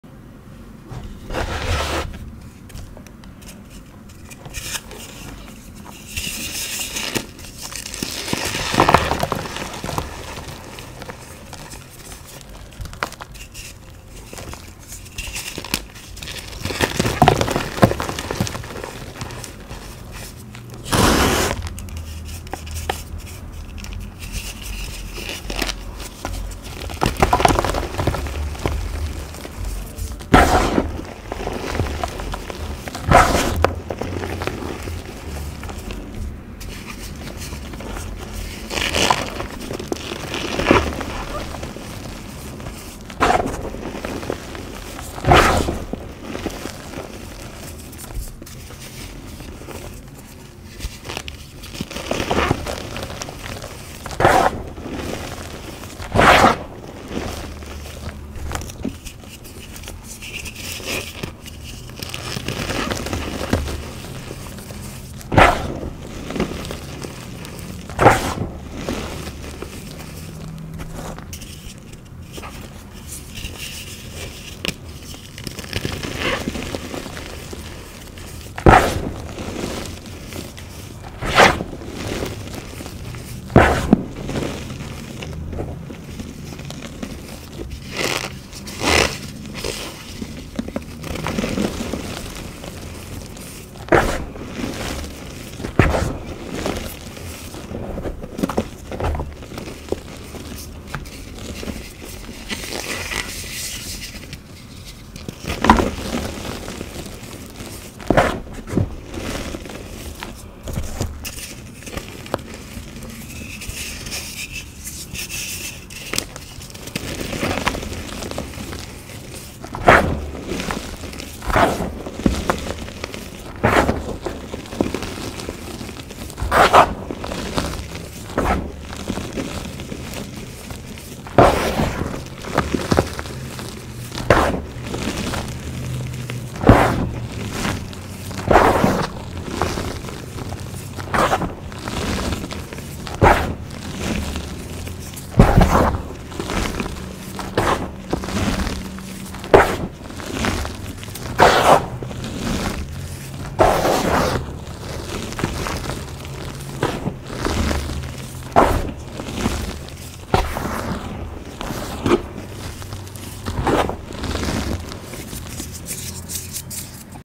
RGC Blocks with Cornstarch | Gym Chalk ASMR | Gym Chalk Reforms | Relaxing | Oddly Satisfying
This started out as a request … but then I added cornstarch 😬
I’ll have to redo the orange and yellow request ☺️